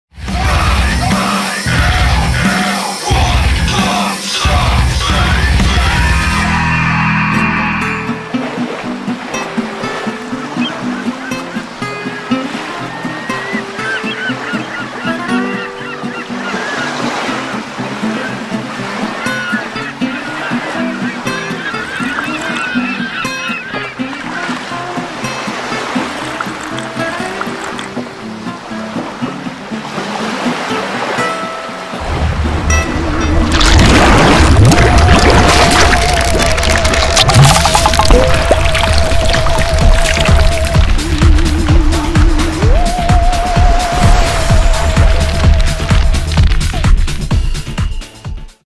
SkullDug Films Presents: Zombie Home Movies - Episode 74 - Paradize!
Frank goes on vacation and parties hardy with and ancient friend!
Horror and dark comedy - Dig the Dark!